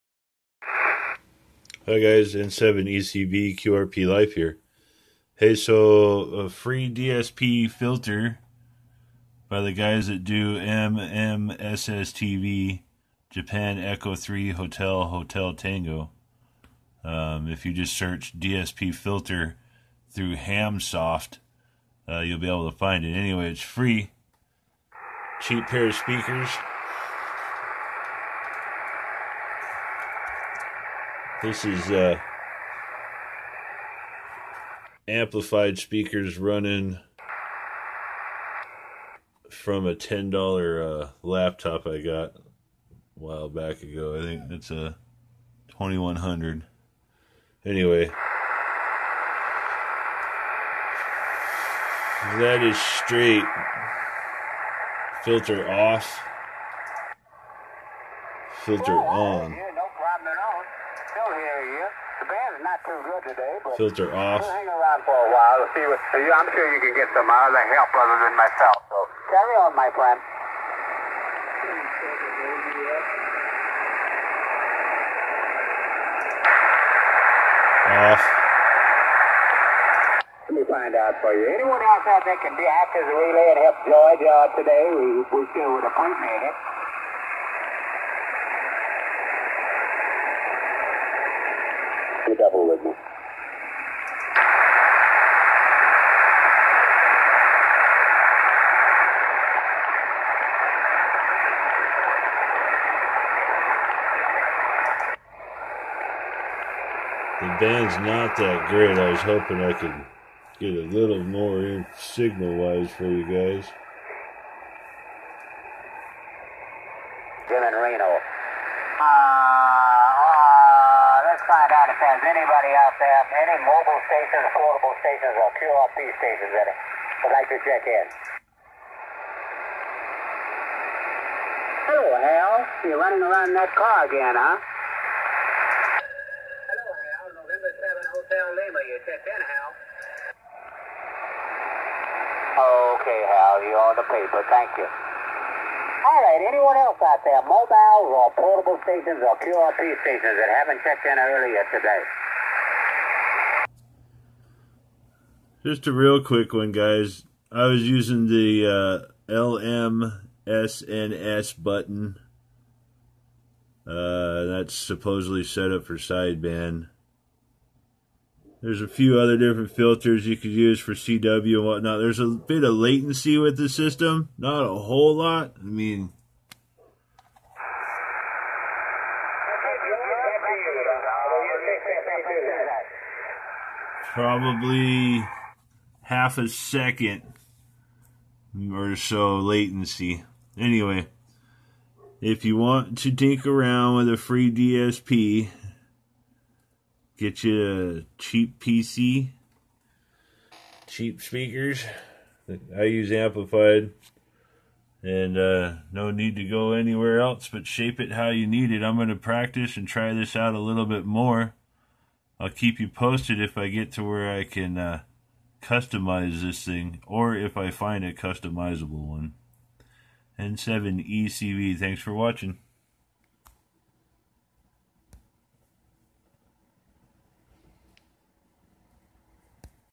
Add Free DSP to ANY Radio !
I used this version because it works well with an old laptop and the latency was minimal.

Pick which one works best for you and start hearing stations!

If, after you install this and start it you hear audio your PC is good enough to try the others.

Using an old AUX cable plug your external speaker output into your PC mic. (use line in)
Wrap any speaker wire with ferrite to eliminate RF
Enjoy DSP and bring in those S1-2 signals